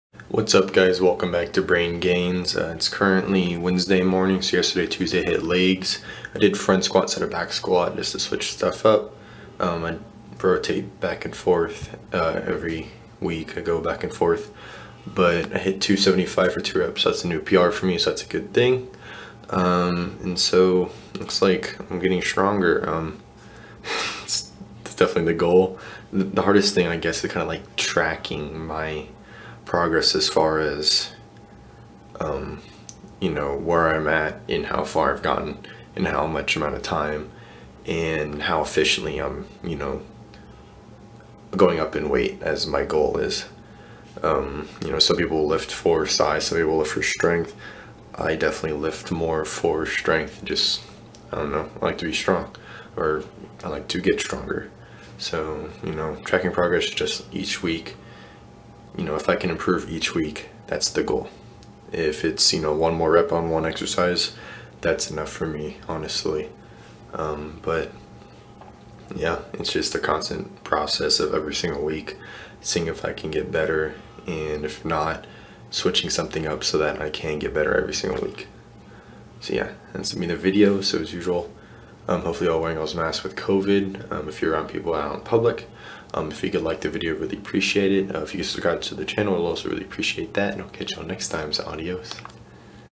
Brain Gains - Leg Day & Tracking Progress
Hit a new PR yesterday with 275x2 on front squat. Slowly making progress towards higher weight.

I'll still be posting my workouts so here is my log of the workout saved on Google Keep:

Workout 9-8-20 (rec)
=7:30pm
-front squat
135*4
225*4
255*2
275*2
265*2
245*2
225*4
135*8
-romanian deadlift
135*6
185*6
225*6
225*4
135*8
-standing calf raises
270*8
310*8
350*8
390*8
390*6
310*8
230*10
-prone hamstring curls
60*4
80*4
100*4
90*4
80*4
60*6
-quad swings
100*6
120*6
150*4
100*8
90*10
80*12
-leg press
135*6
225*6
315*6
405*4
405*4
225*8
=8:45pm

I've only added EDM/Dubstep to it so far but there will eventually be a lot more music from multiple genres